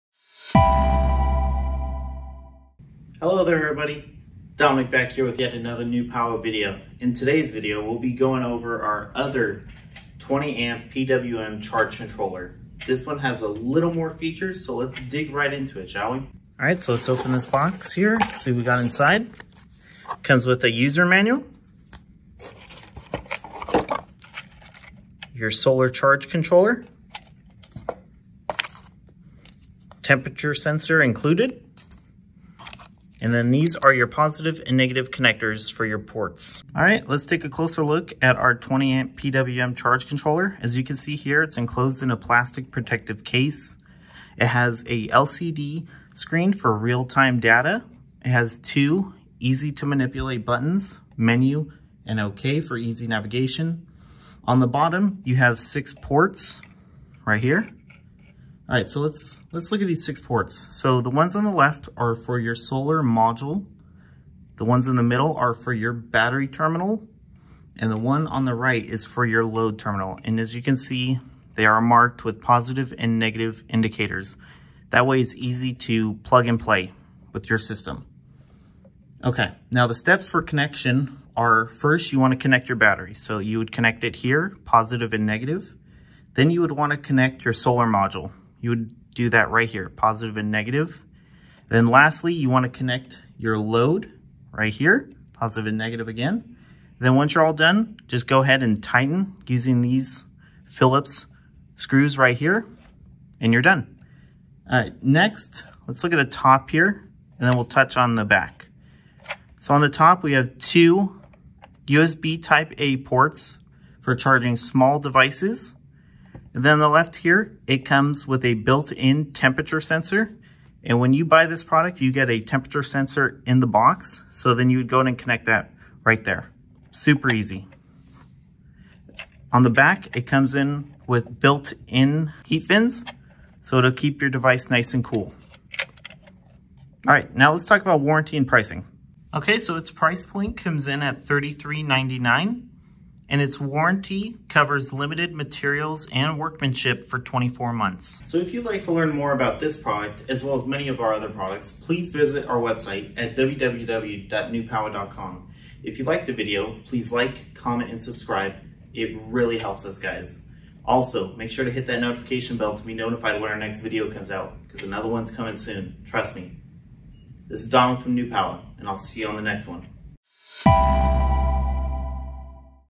Newpowa’s 20A PWM Solar Charge Controller
In this Video, Domenic will be going over our 20A PWM Solar Charge Controller (SKU: NPA-MAX20EU). If you would like to learn more about this product,

-- Timestamps --
0:00 Intro
0:17 Unboxing
0:36 Closer Look
1:19 Steps for Connection
1:44 Other Features
2:17 Warranty & Pricing
2:28 Closing